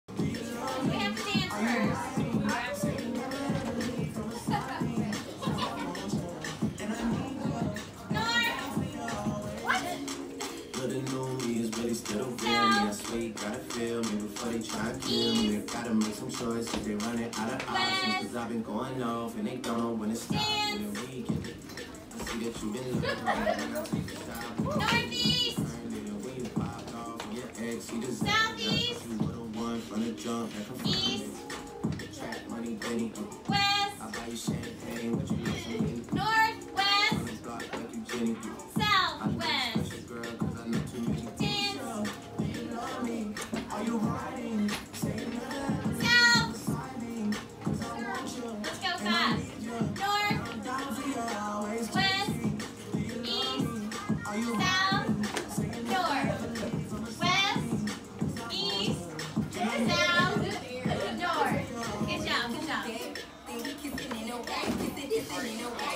Kinesthetic and Musical Learners
Mrs. Lotze's 5th grade Social Studies class enjoying music and movement.